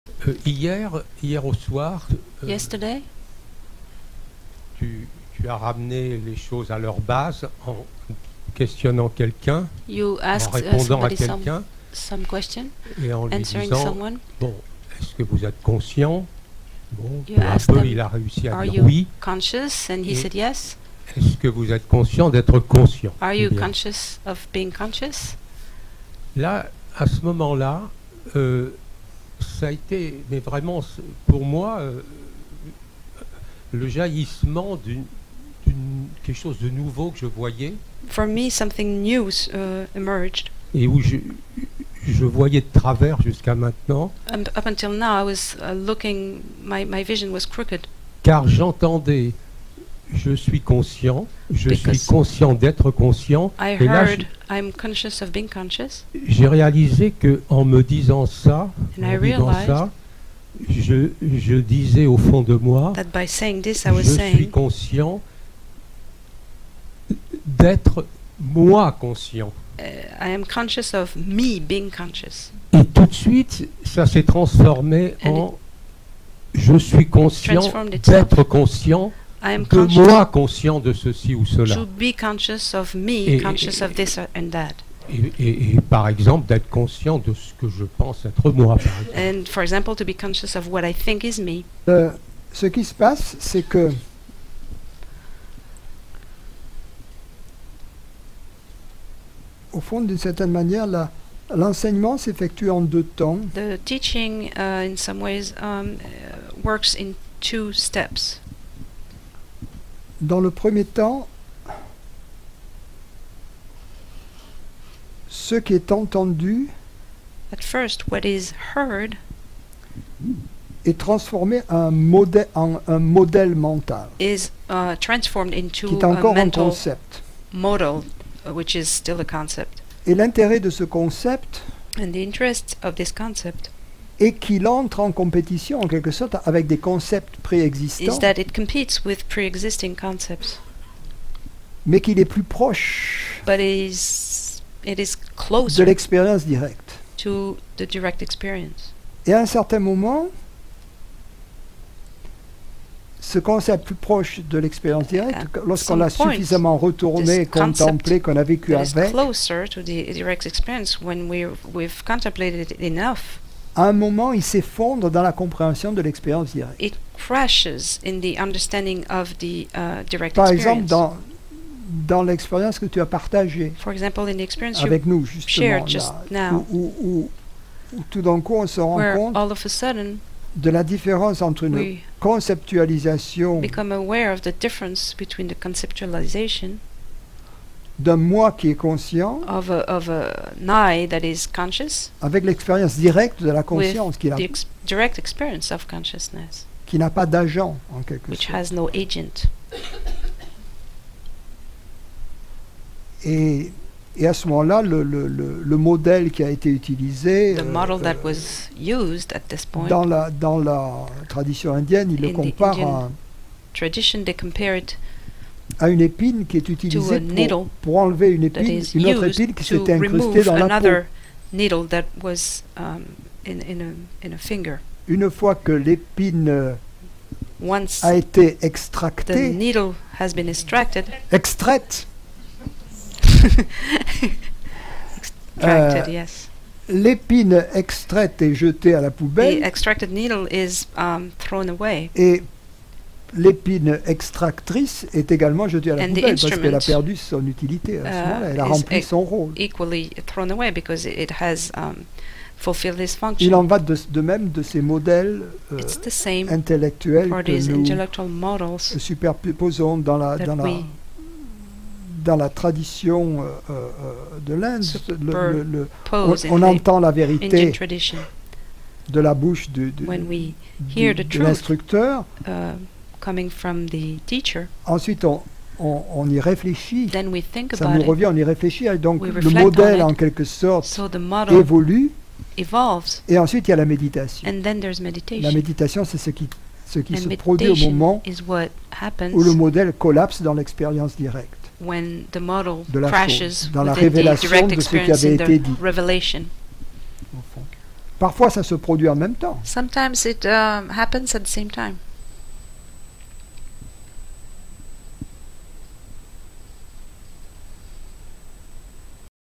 Francis Lucille - Le concept et l'expérience directe
Francis décrit deux étapes successives du chercheur de vérité, la première où les paroles de l'enseignant constituent un nouveau concept, puis la seconde où ce concept se dissout dans l'expérience directe, le rendant désormais inutile.

Francis Lucille à Paris, déc.2012
Français - English